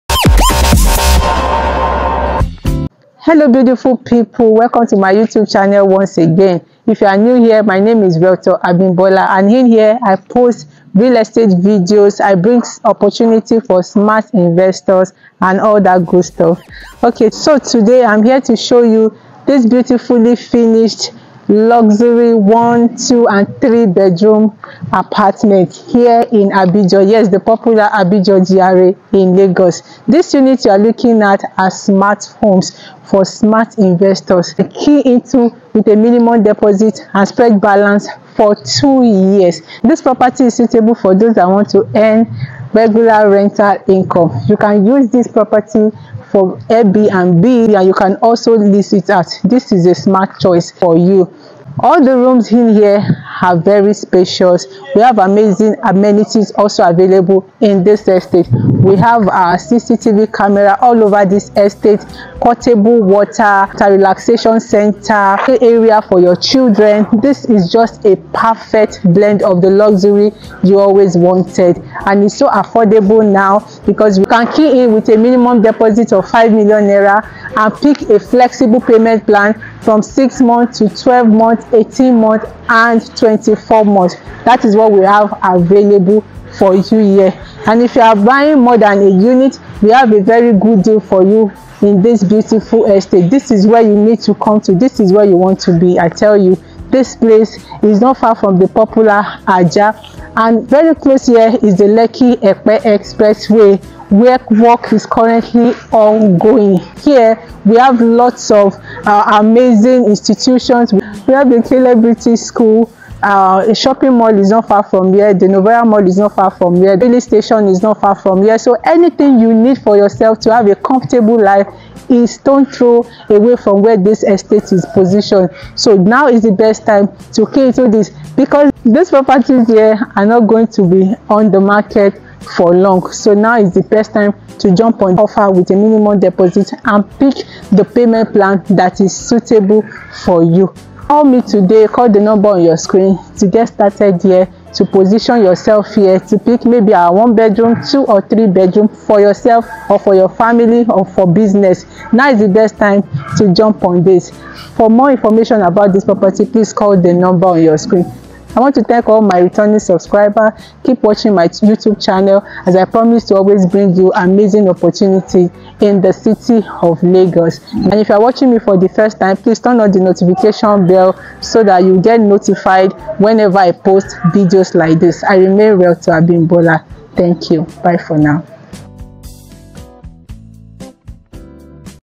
"Inside Lagos' Luxury Smart Apartments in lekki axis
This a Luxury apartment specially built for you; a perfect choice for your Airbnb business and has superb designs that also give you value for your money. The Location and Ambience of this estate in Abijo are quite impeccable with easy accessibility. This property boasts of State-of-the-Art Finishing.

You stand a chance to enjoy the advantage of beautiful architectural masterpieces which are a seamless blend of class and style

Apartment Types
1 Bedroom Apartment:38m
2 Bedrooms Apartment:48m
3 Bedrooms Apartment-53.5m
Outright payments (0-6months)

Flexible payment plans for up to 24 months.

Won't you rather come and inspect this beauty to make an informed decision?